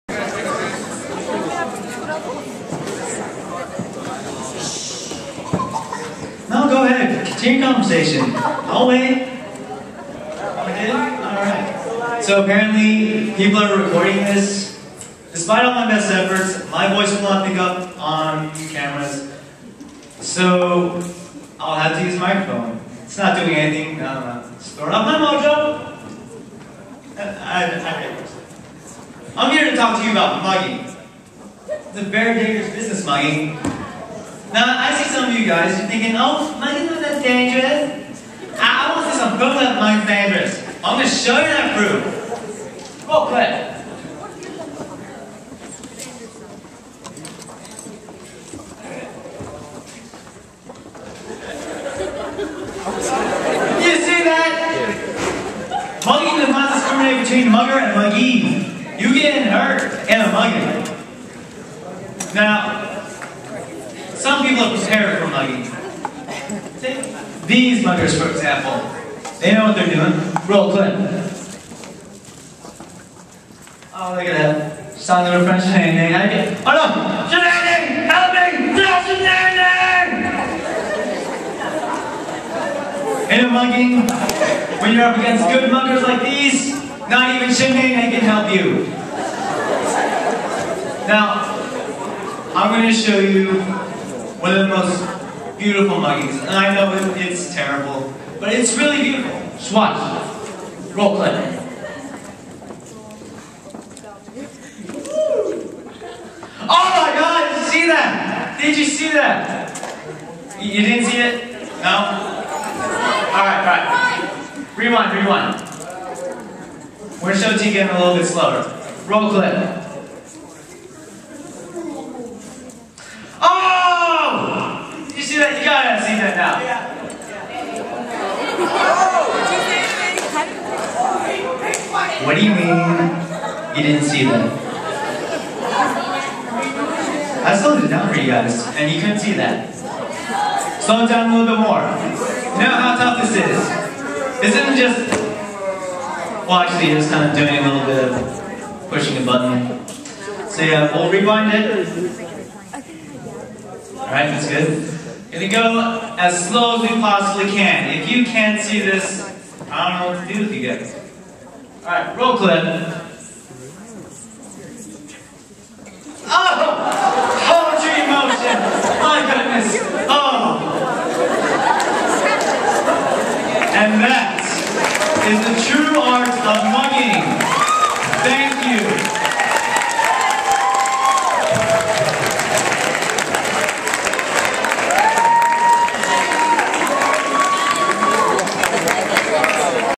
The Art of Mugging
This short video is a skit done by young people from the Lake Highlands Ward.  Calvin is the narrator.